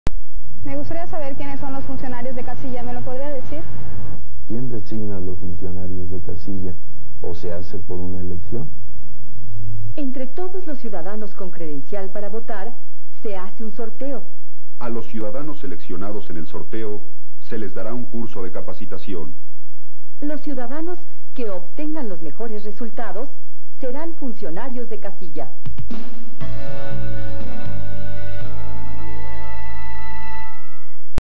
Comerciales mexicanos: IFE 1991 4
Creación del sorteo y la capacitación para funcionarios de casilla